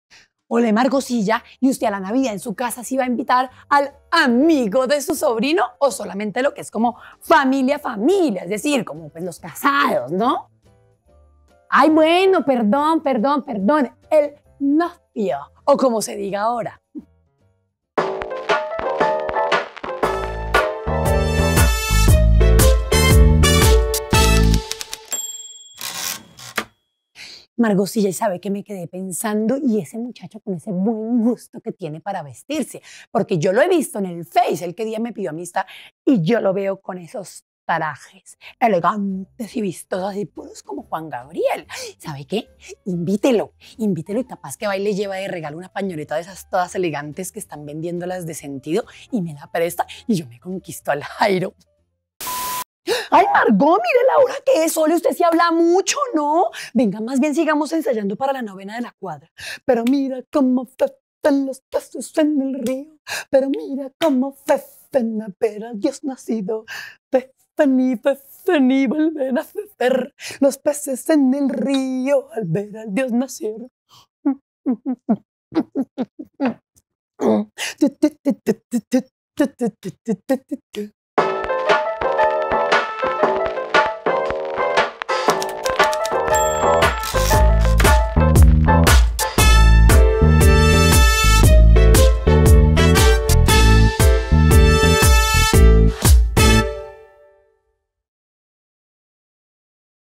La Tía Nohora y "el amigo" del sobrino
La Tía Nohora creció en un entorno conservador y tradicional, lo bueno es que siempre está dispuesta a aprender... Así sea por conveniencia.